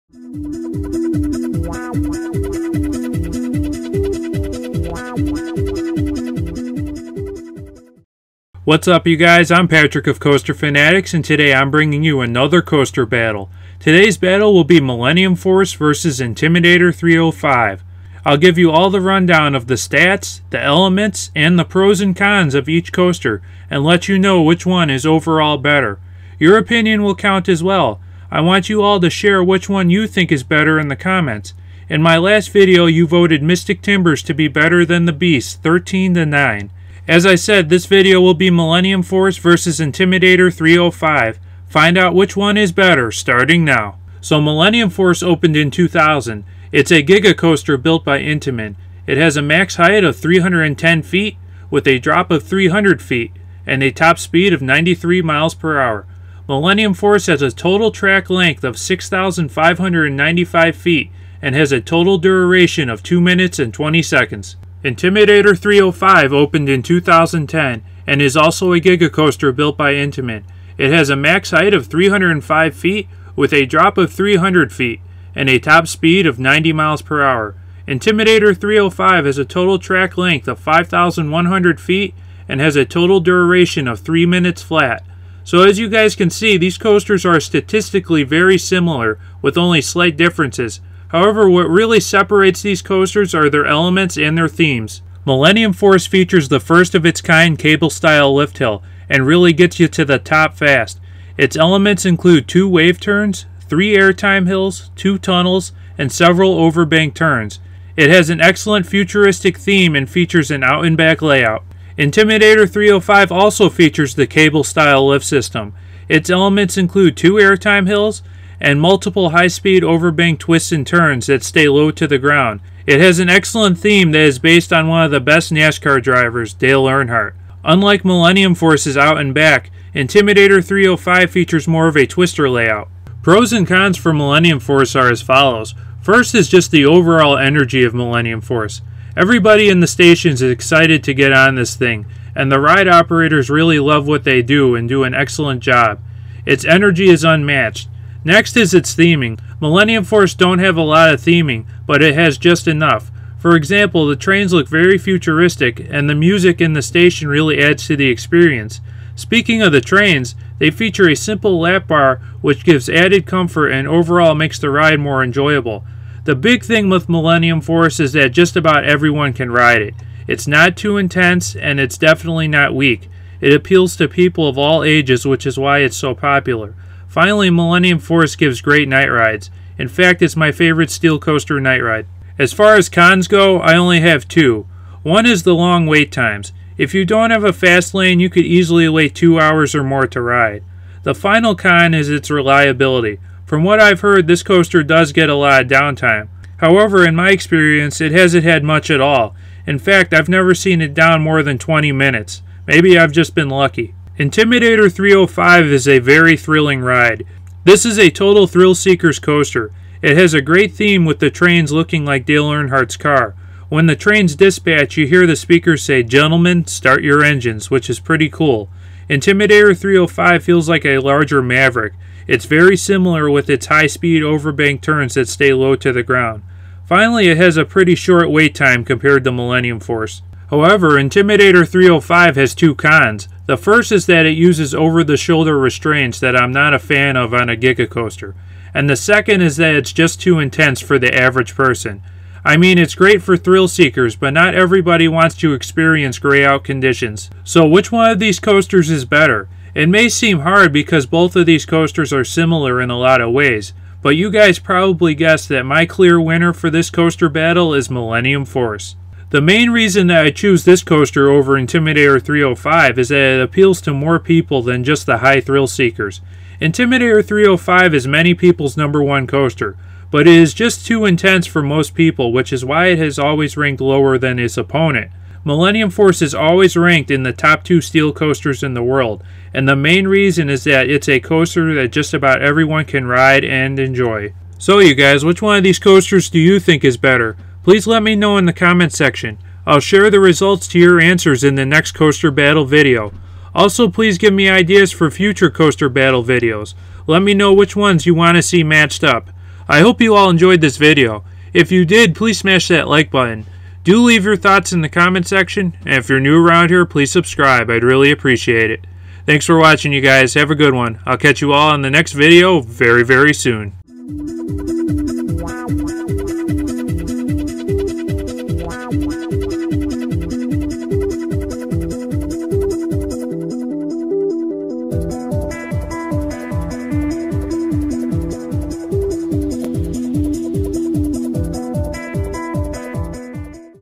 Millennium Force vs. Intimidator 305 Roller Coaster Battle! Which One Is Better?
Hey Coaster Fanatics, Today I'm Bringing you another coaster battle. In this comparison it will be Millennium Force vs. Intimidator 305. I will tell you which one is better and why and will give the pros and cons for both of these coasters. Make sure you let me know which one you think is better. Thank you and ride on!

The votes are in and you guys share a different opinion than mine. You voted Intimidator 305 to be better than Millennium Force 28 to 17!!!